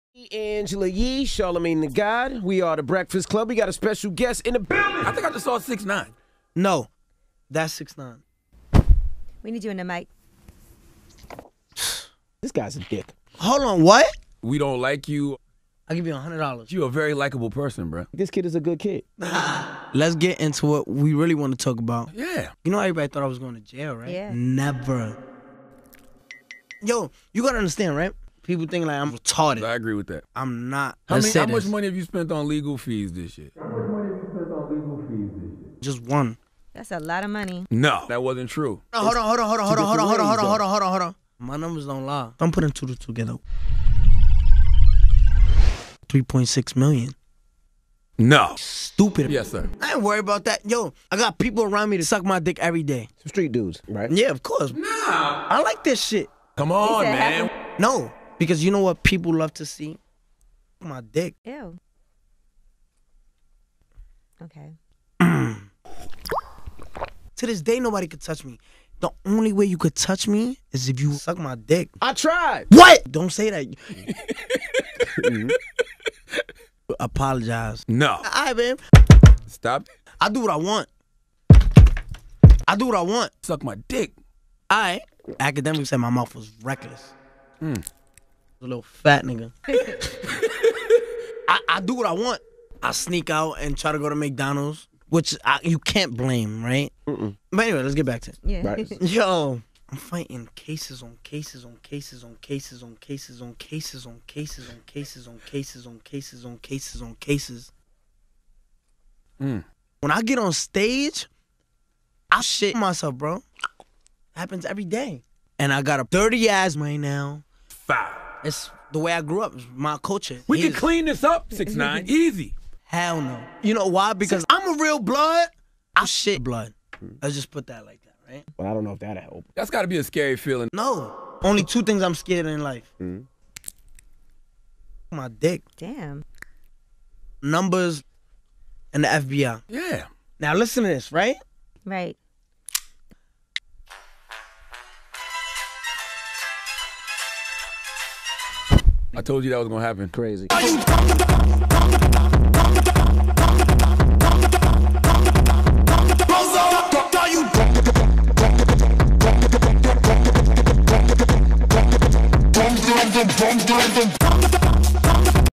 6IX9INE Loses His Cool Explaining His Legal Battle Against the Feds!
6IX9INE Loses His Cool Explaining His Legal Battle Against the Feds!